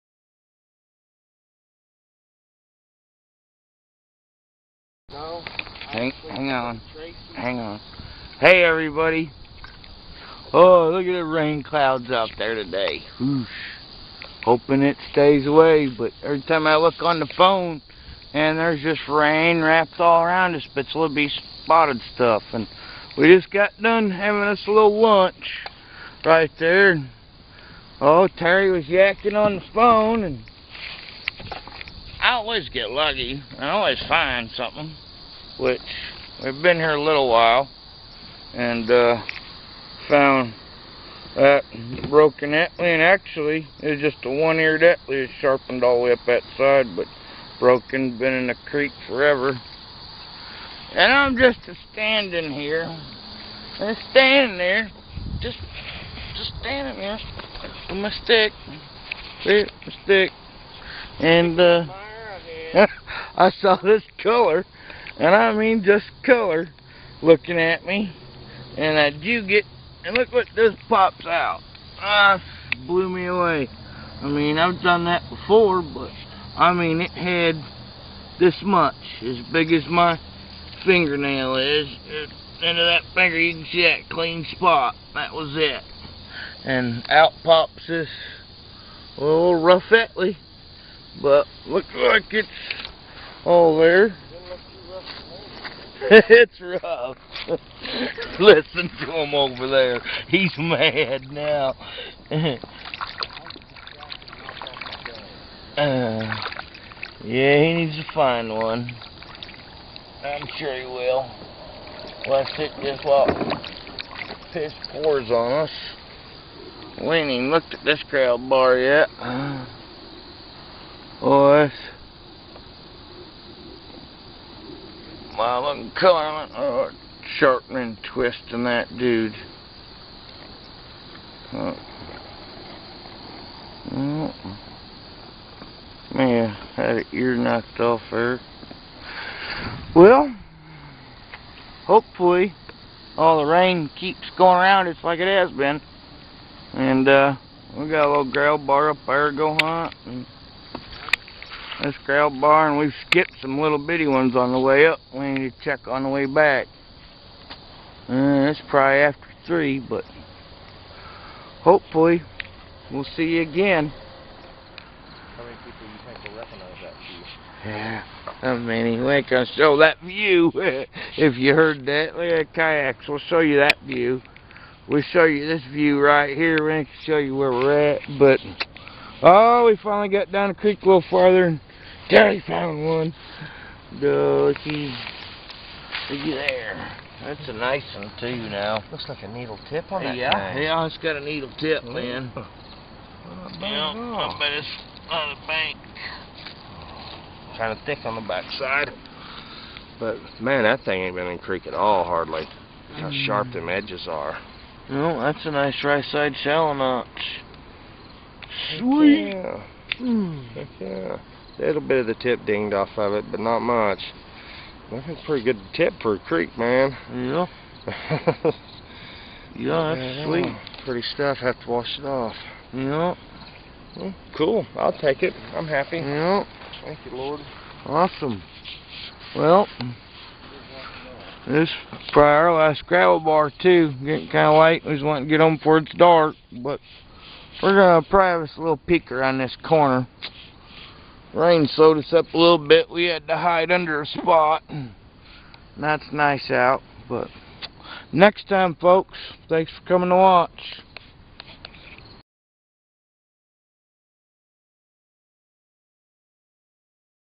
new waterway 9-10-11.mpg
Terry & I out on a new waterway, alwaz something to find & need to go back & go much father Up then where we where